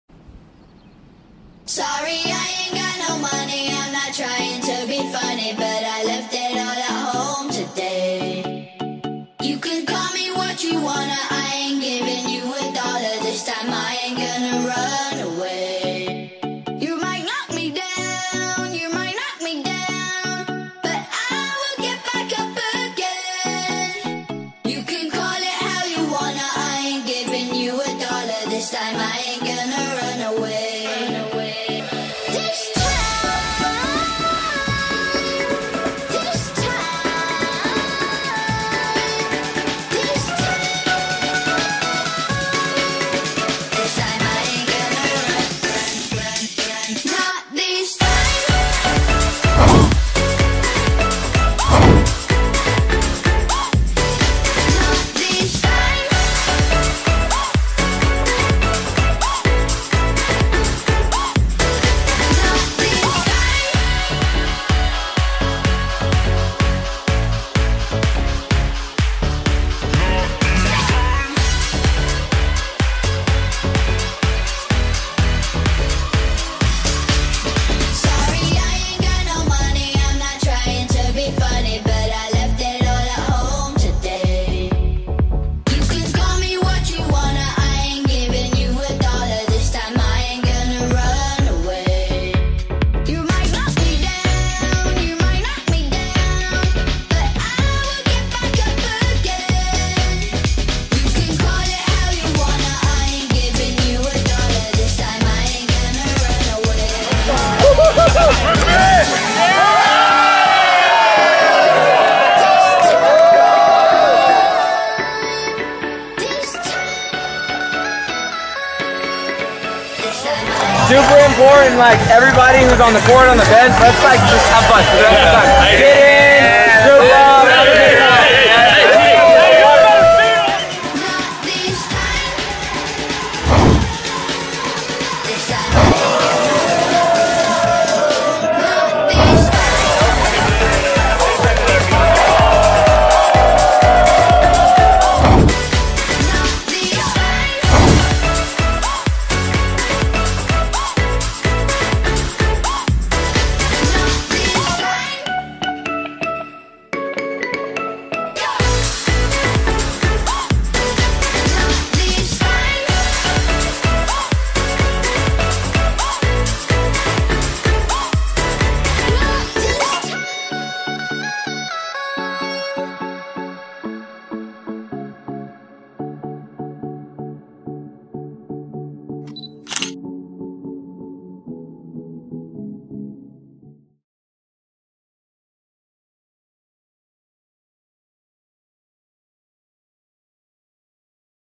ASICS World Series of Beach Volleyball - Team Sunny Livin (6-Man 2016)
Official Recap of Long Beach 6-Man x Team Sunny Livin

Special thanks to our players, friends, and family for continuing to support Sunny Livin Company. Our 1st year competing in the World Series of Beach Volleyball 6-Man was an unforgettable one. We matched up against heavy competition and found us finishing in 2nd place. Again, it was an amazing event put on by ASICS and we can not wait for next year!

2016 SUNNY LIVIN ROSTER:

1) Kris Johnson - M
2) Michael Brinkley - L
3) Jace Olsen - OH
4) Chris Austin - S
5) Ryan Redira - L
6) Dane Worley - OPP
7) Taylor Hammond - S
8) Dilon Emery - OPP
9) Charlie Condron - L
10) Garret Komo - L
11) Matt Evans - OH
12) TJ Defalco - OH
13) Kevin Campbell - M
14) Dilon Auxier - OH
15) Aniefre Thomas - M
16) Myles Muagututia - OH
17) Relyea Speller - OPP